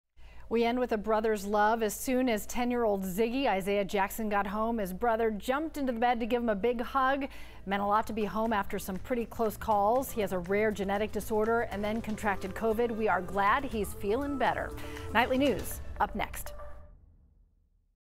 Child who battled rare genetic condition, COVID back home
Being home means a lot after long stretches in the hospital some pretty close calls.